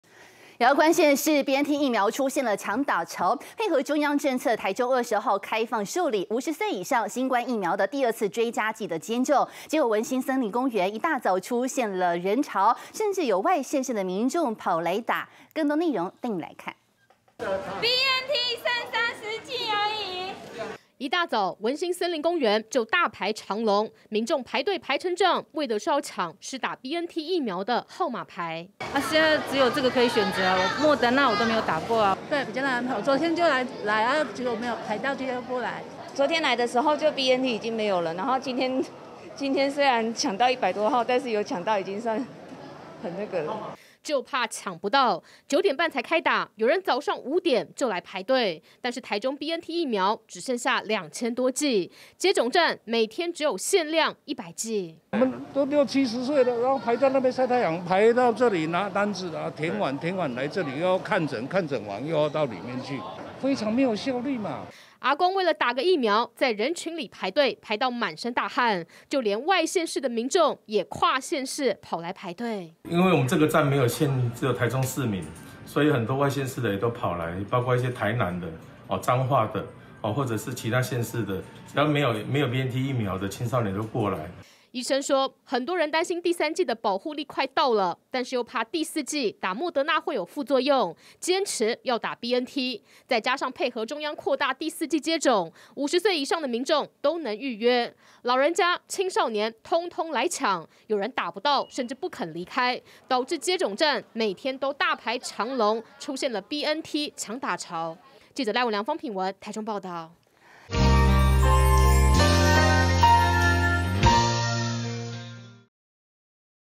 第四劑開打！ 台中現BNT搶打潮 每日限100劑民眾大排長龍
BNT疫苗出現搶打潮，配合中央政策，台中昨（22）日開放受理，50歲以上新冠疫苗的第二次追加劑接種，沒想到文心森林公園，一大早就大排長龍，甚至有外縣市的民眾跑來打疫苗，由於青少年也可以接種，而民眾也擔憂施打莫德納會有副作用，因此接種站每天都大排長龍，甚至有人沒打到疫苗也不肯離開。